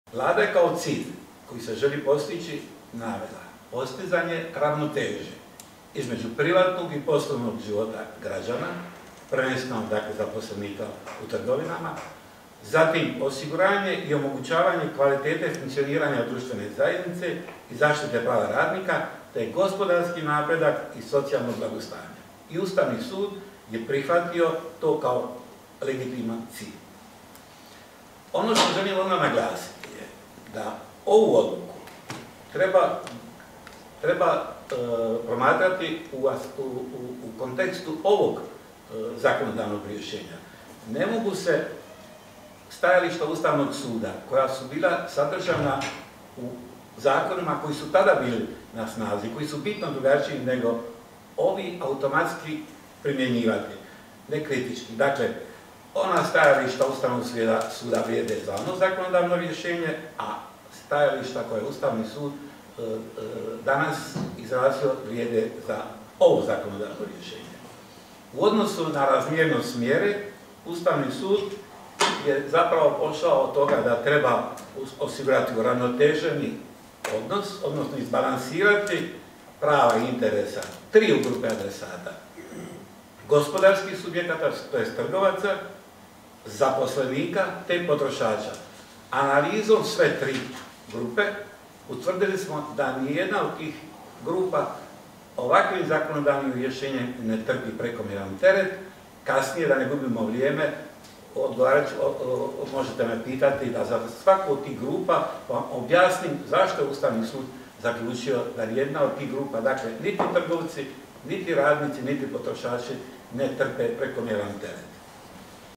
Šeparović pojasnio odluku o zabrani rada nedjeljom
Predsjednik Ustavnog suda Miroslav Šeparović  održao je konferenciju za medije.